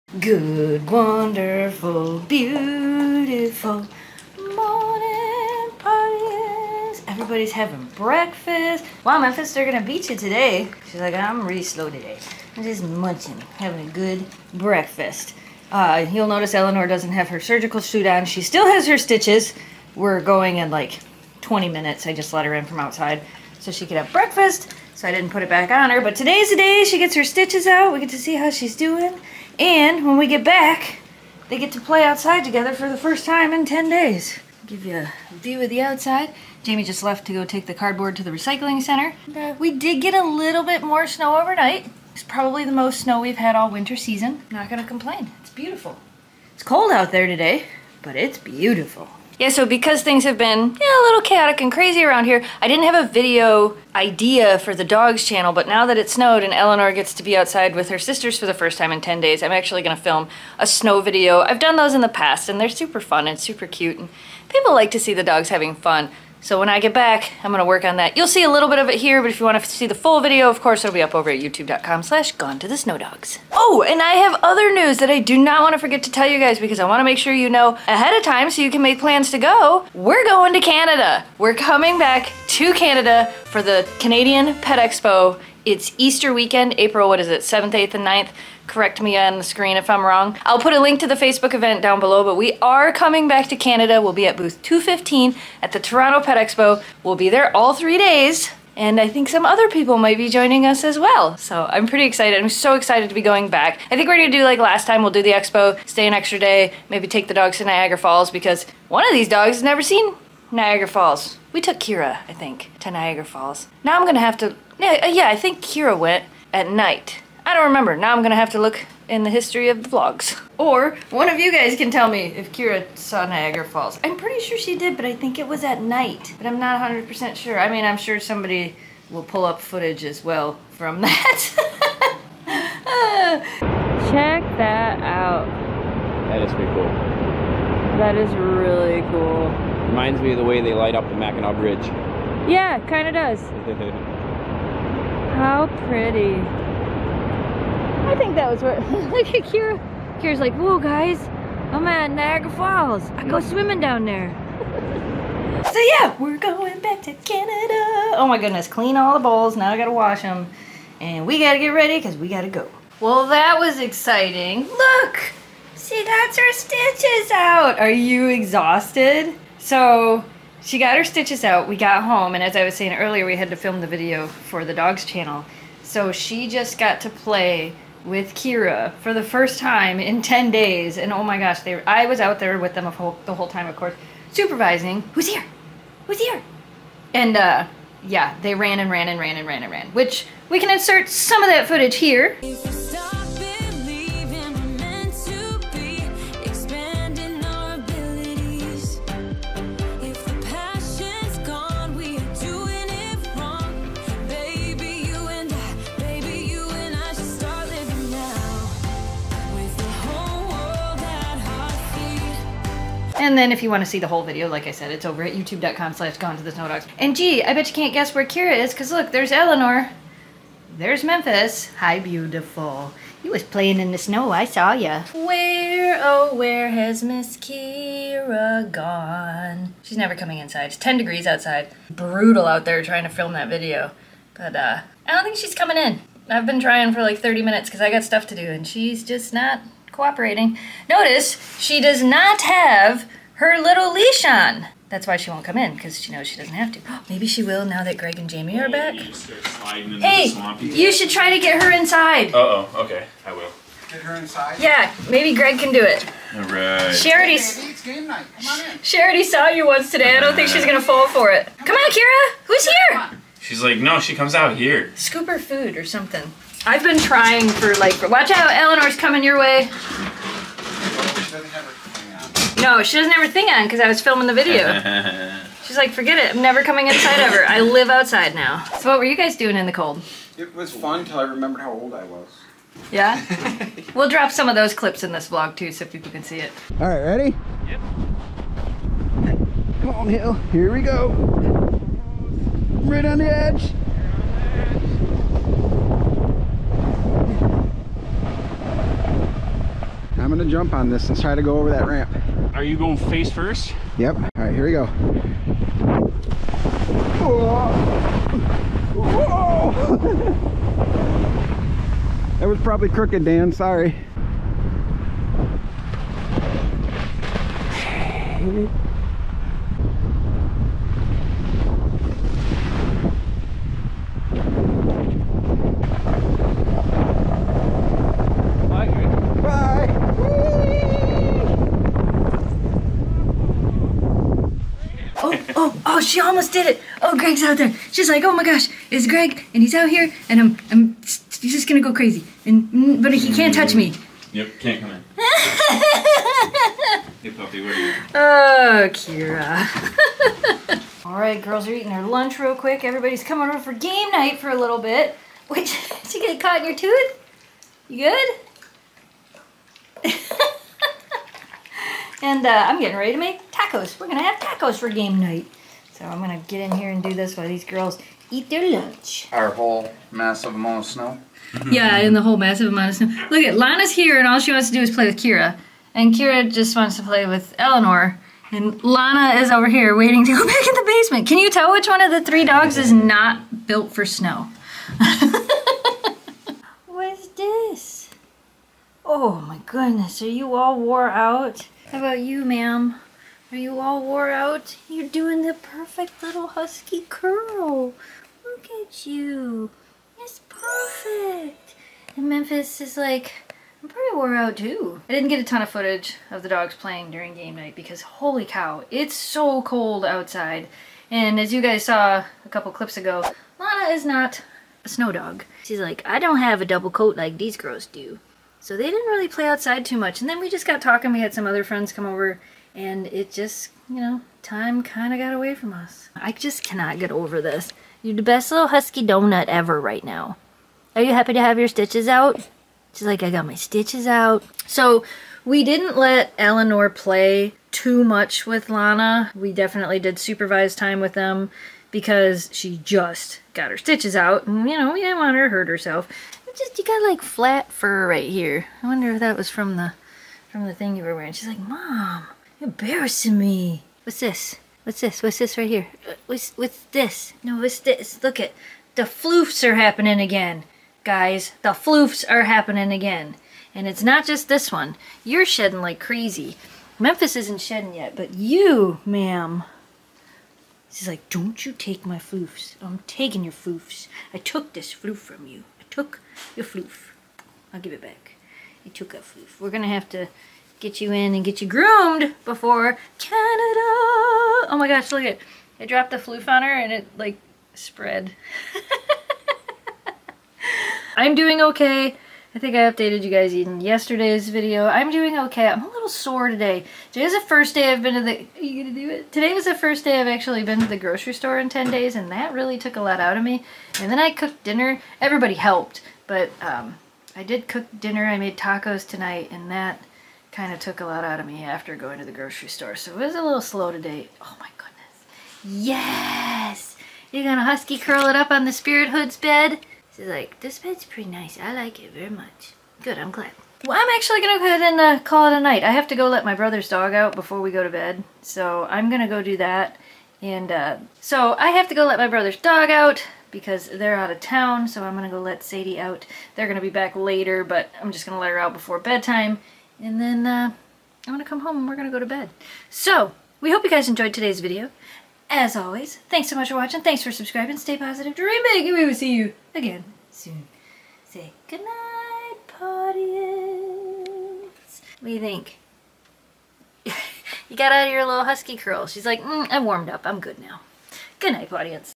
My Huskies Get to PLAY Again! No More Stiches!
Eleanor gets her stitches out today!  Now she still has to not be as crazy and play and stuff just yet,  but we did let them play and monitored them a bit.  :)

⬆⬆ The Coloring Book Link is IN the Beacons Link  ⬆⬆